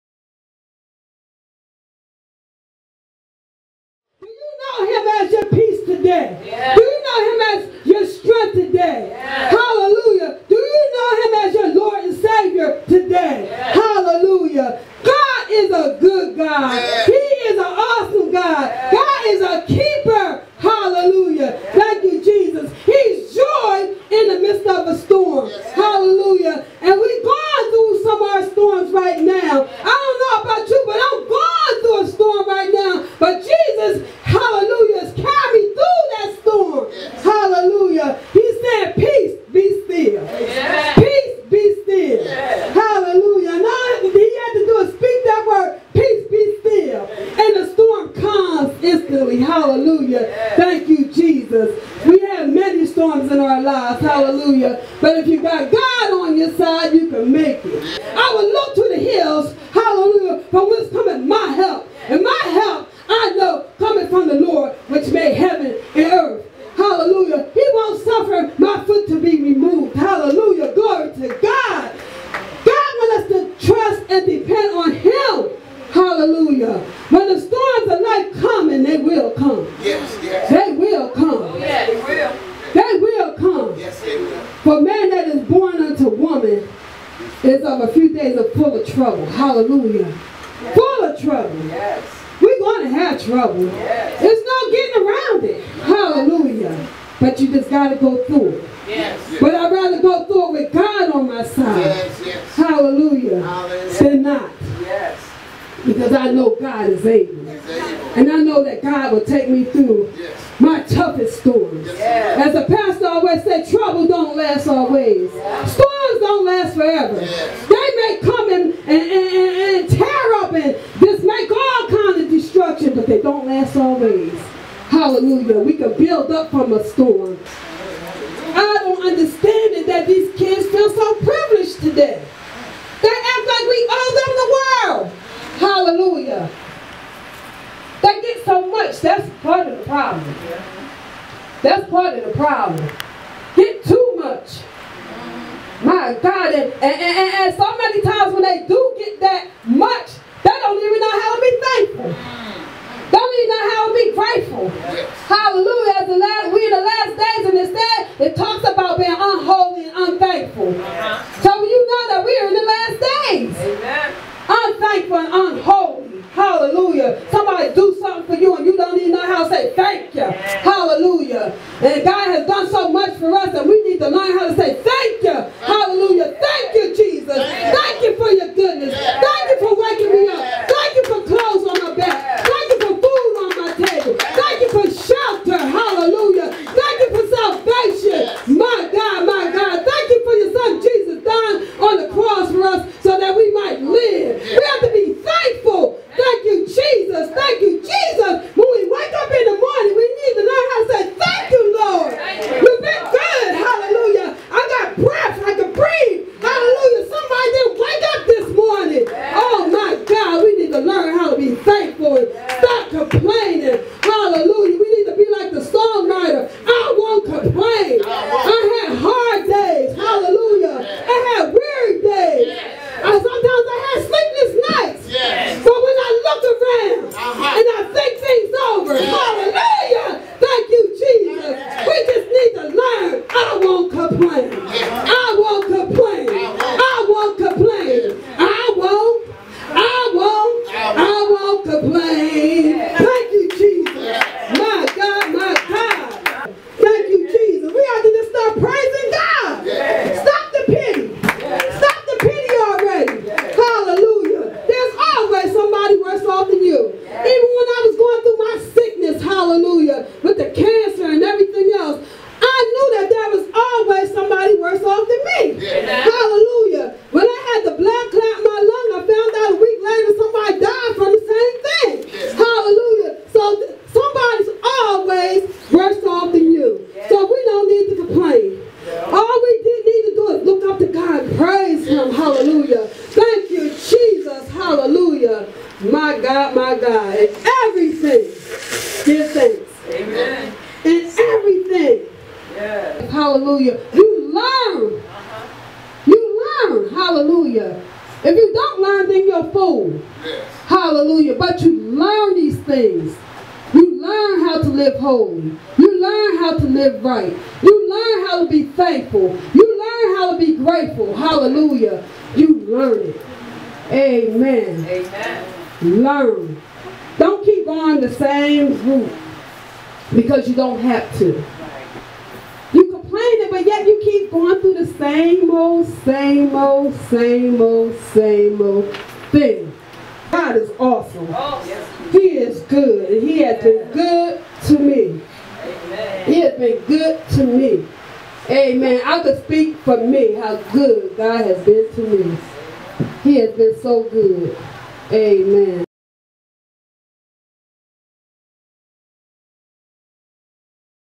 Tracy Brice Message
Co Pastor Tracy Brice Spreading the Gospel and her Testimony at "Christ Temple Of Love"(F.A.P) 504 Edgemont Ave Chester, PA Sunday Morning Services 10:30am Sunday Mornings